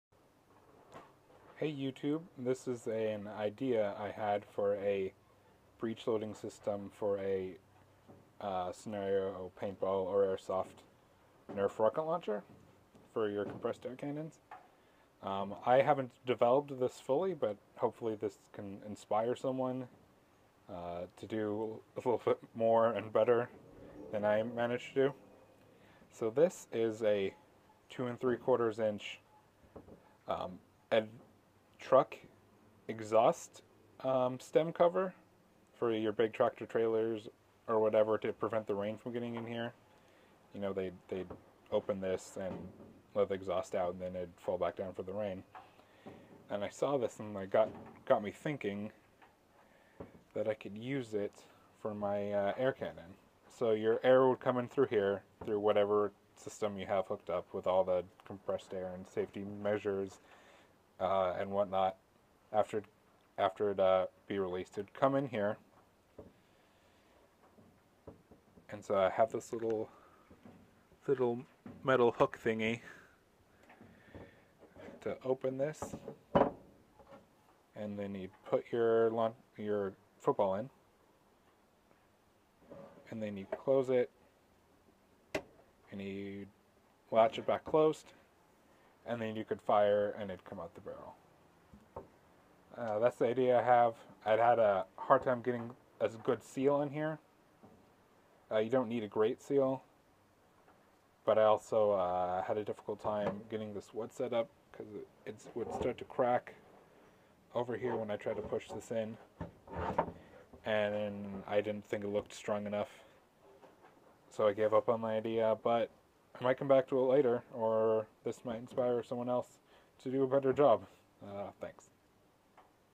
Rain Cap Breech Loading Nerf Rocket Launcher Idea
This is an unfinished design I worked on for a breech loading mechanism for a Paintball/Airsoft Nerf football "rocket" launcher. It uses a  2-3/4" Exhaust Rain Cap that I bought at Tractor Supply Co. The other parts I had lying around my workbench. I could never get a setup that I was happy with. Hopefully someone will take this idea and make it better.